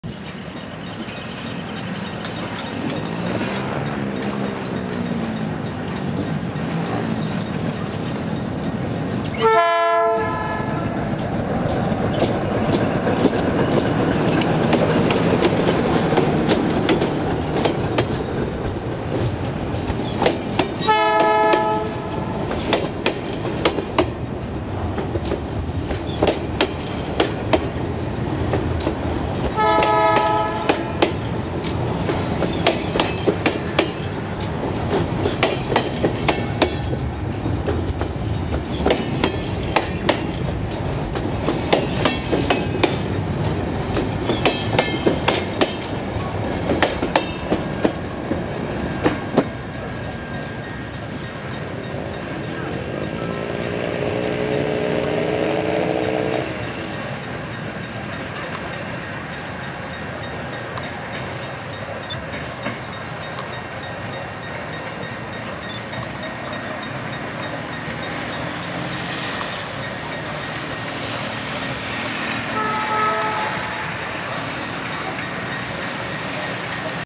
6258 PASANDO POR LIMA.
GAIA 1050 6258 PASANDO POR LIMA CON UN TREN DE 10 PC'S PROCEDENTE DE PATIO PARADA EN ROSARIO CON DESTINO TERMINAL ZARATE.

VIDEO: FEDERICO ILLENZEER.